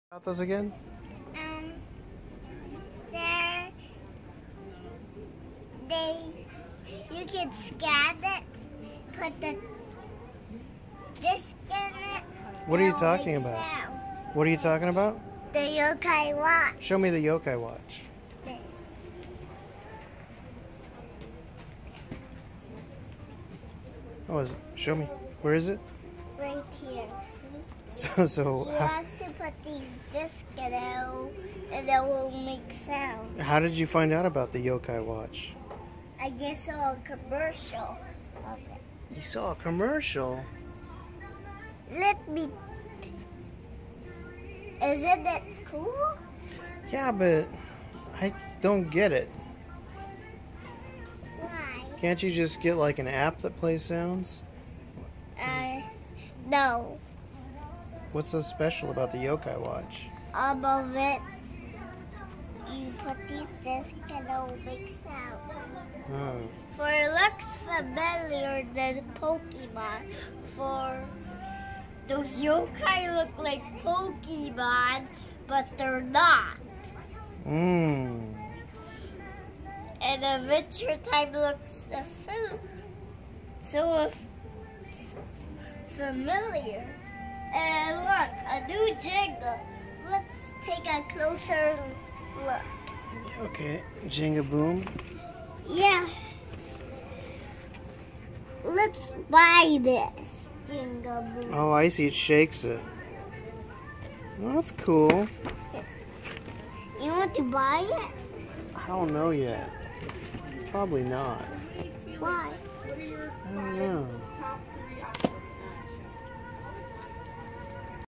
Carlyle explains the Yo-kai watch
Carlyle spots a Yo-Kai watch in the store and explains it to his dad.